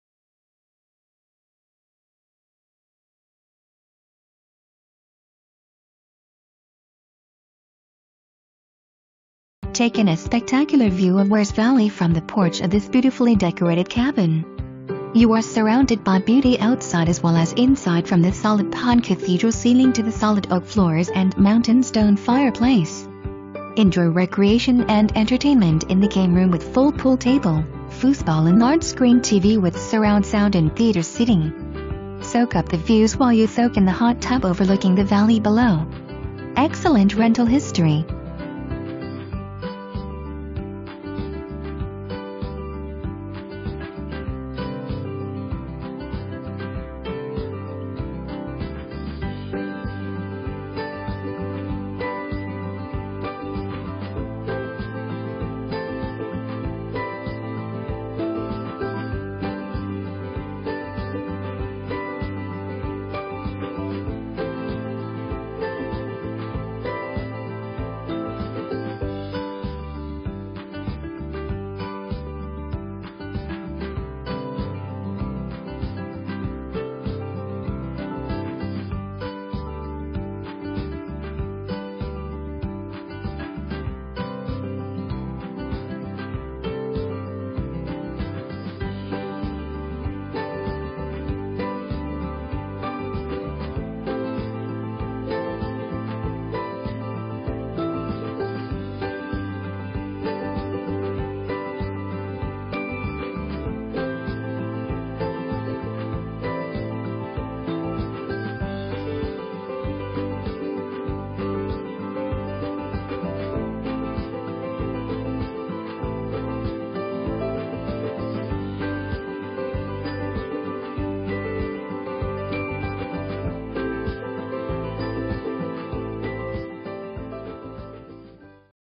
3204 Massey Road
3 bed, 3.0 bath, 2,160 SF, MLS# 200602

Take in a spectacular view of Wears Valley from the porch of this beautifully decorated cabin. You are surrounded by beauty outside as well as inside from the solid pine cathedral ceiling to the solid oak floors and mountain stone fireplace. Enjoy recreation and entertainment in the Game Room with full pool table, Foosball and large screen TV with surround sound and theater seating. Soak up the views while you soak in the Hot Tub overlooking the valley below. Excellent rental history!

Presented By:
Mark Booth